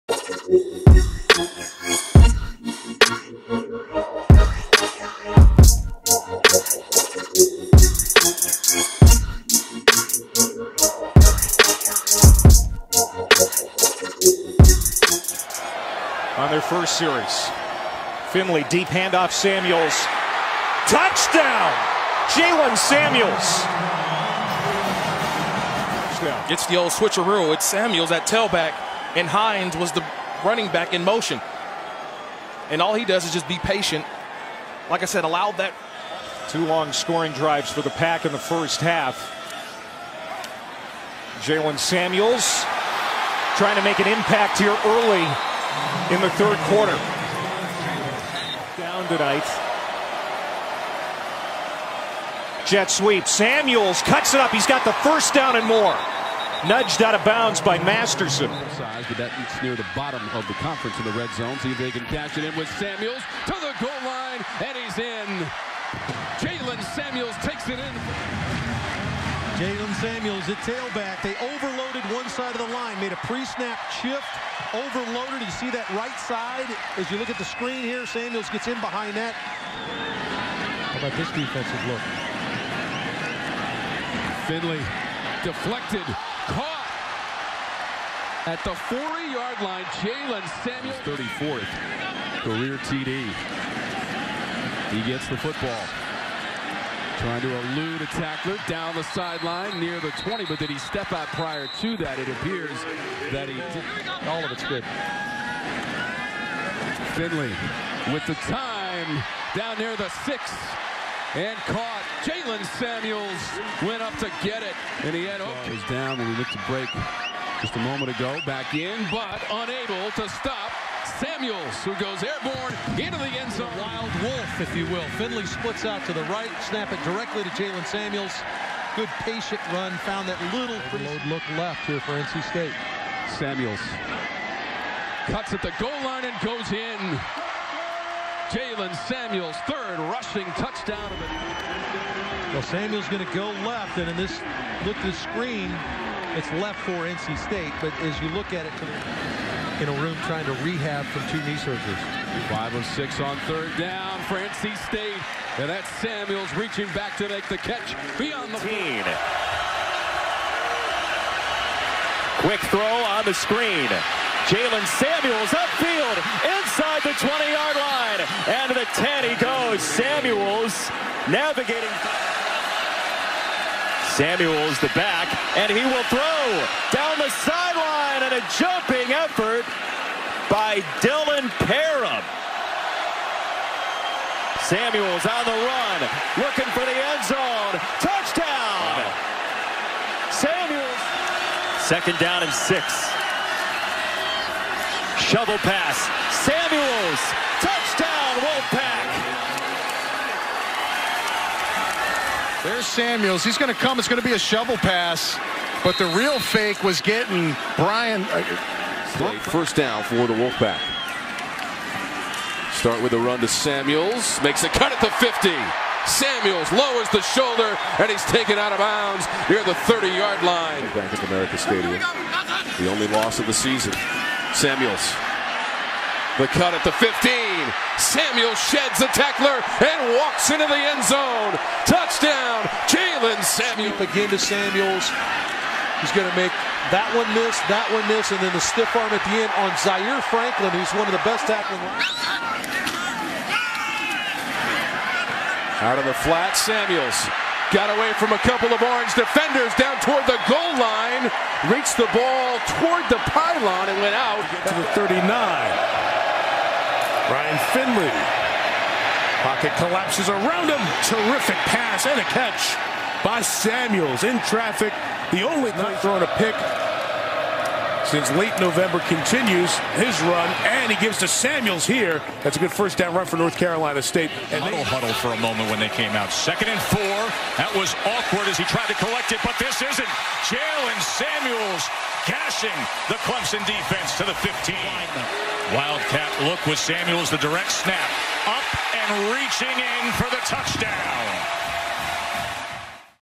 Jaylen Samuels NC State 2017 Season Highlights ᴴᴰ || "Welcome to Pittsburgh"
Highlights from NC State WR/HB Jaylen Samuels 2017 season.